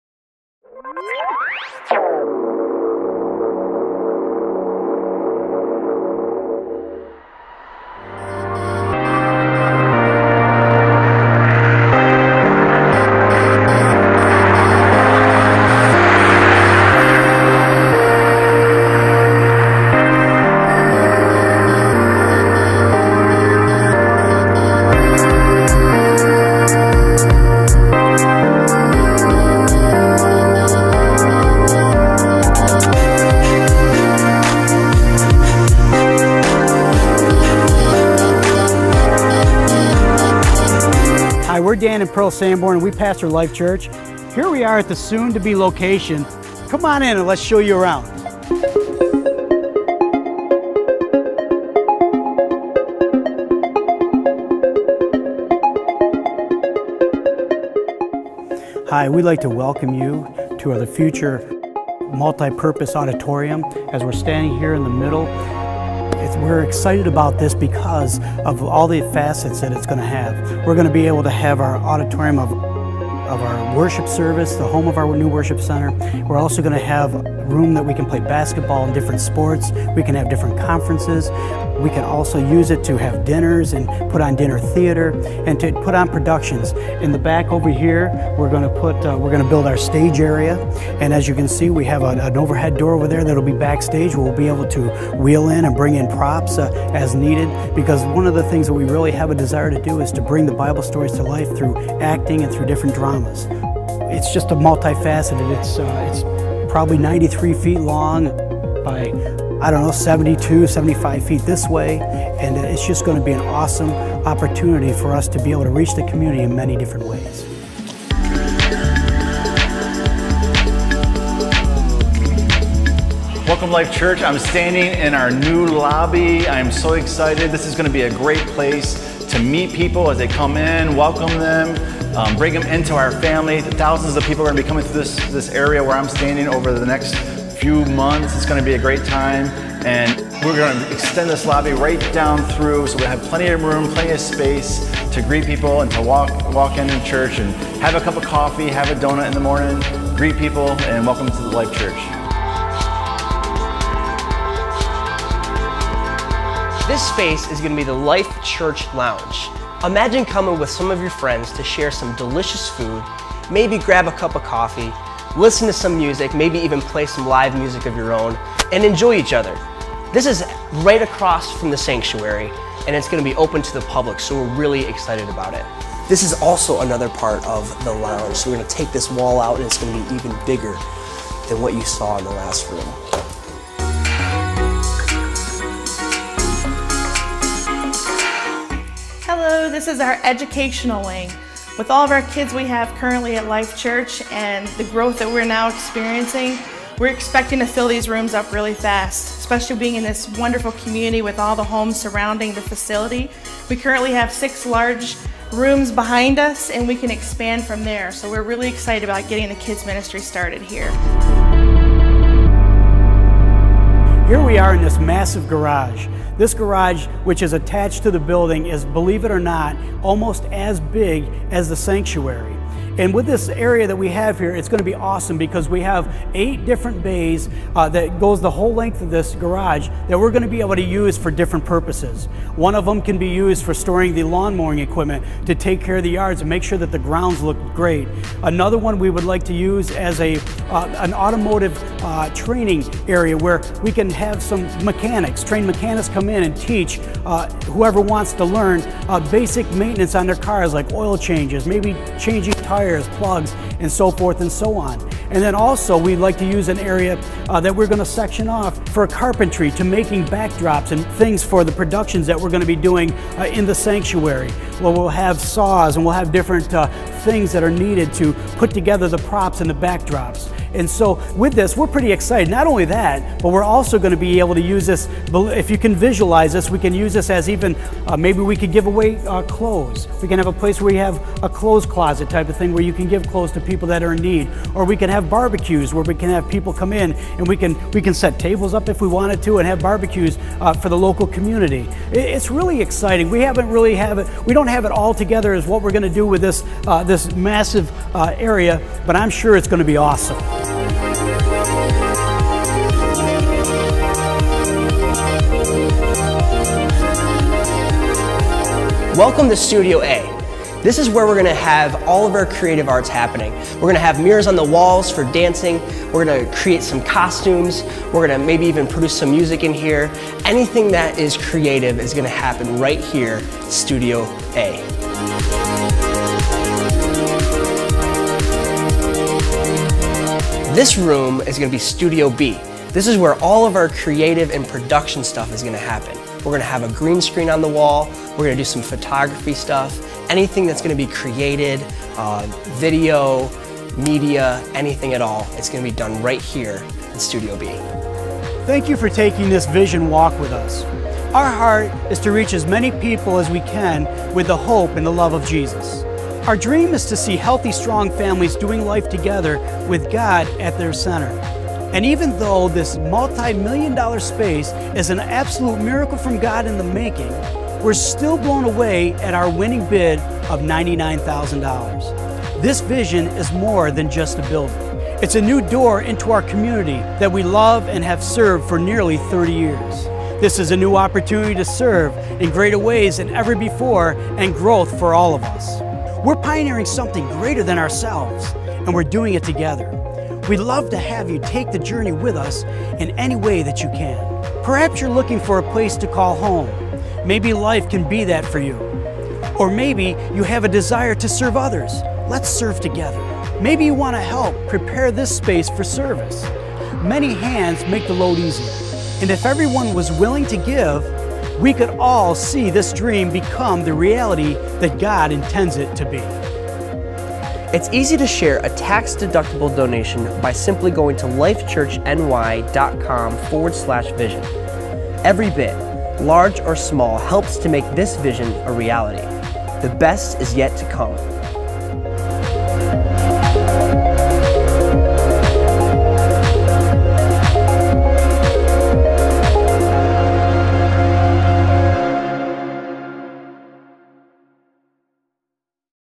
Life Church Vision Video 2014
We're excited that God has opened the door for us to move to the center of our city! Even though we have served Rome for nearly 30 years, we believe that this is just the beginning of what God is going to do! You're getting a chance to see our vision for the new location of Life Church.

God is doing great things!